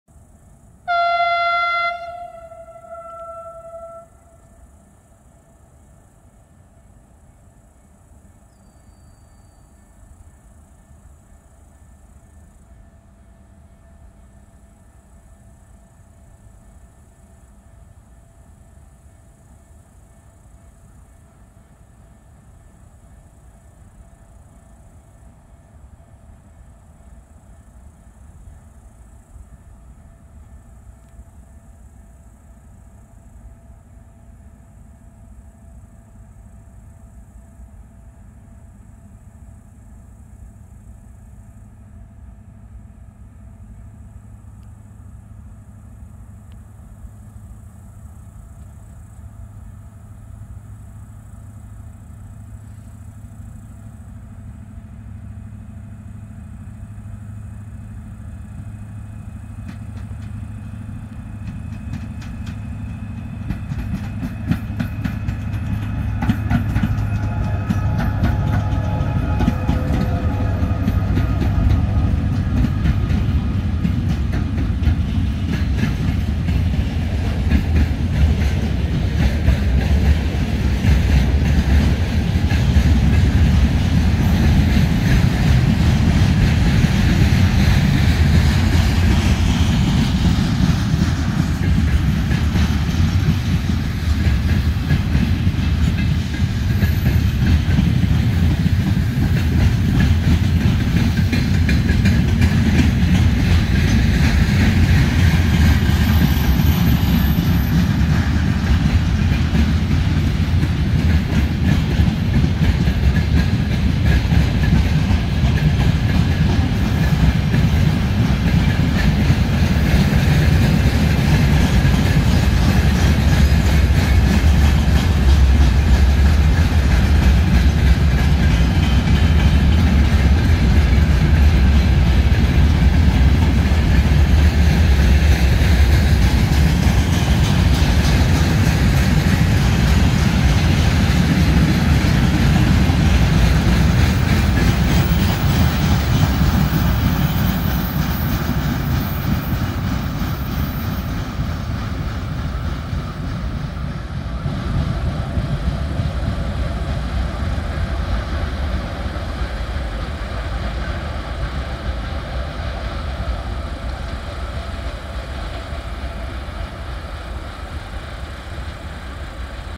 LTG- Cargo three Siemens Er20cf
Lithuanian railways (LTG- Cargo) with a freight train three Siemens Er20cf between Gaižiūnai- Jonava station. Lithuania at night trainspotting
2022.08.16